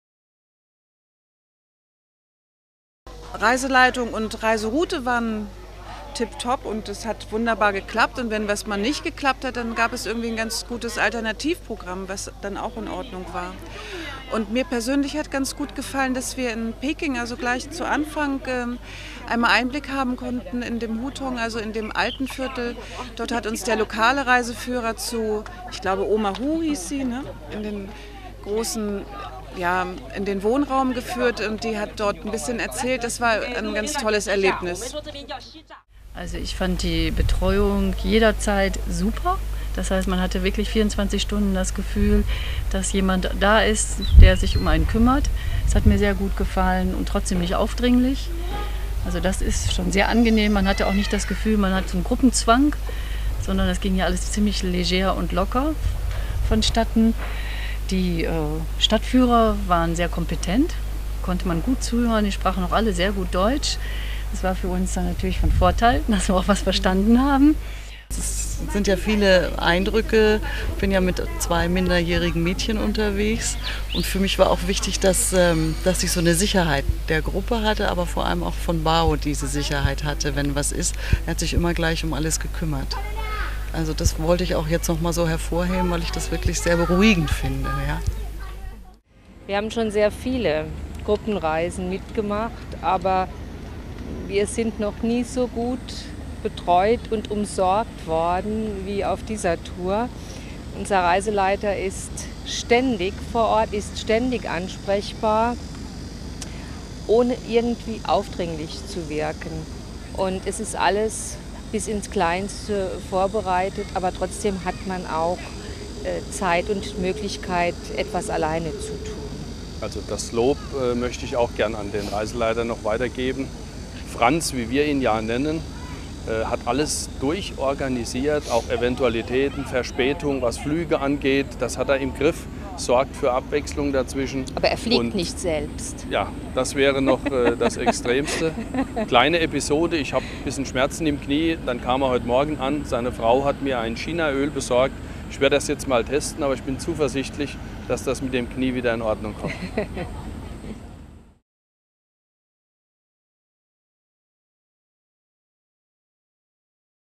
China Tours - Über die China Tours Reiseleiter
Mehr über unsere Reiseleiter Feedback ist uns wichtig. Vor allem wenn es um unsere Reiseleiter geht, die bei China Tours einen speziellen Mehrwert darstellen. Deshalb haben unserer Gäste ihre ganz persönlichen Eindrücke zu den Reiseleitern in China bei mit uns geteilt. Hier eine kleine Zusammenstellung.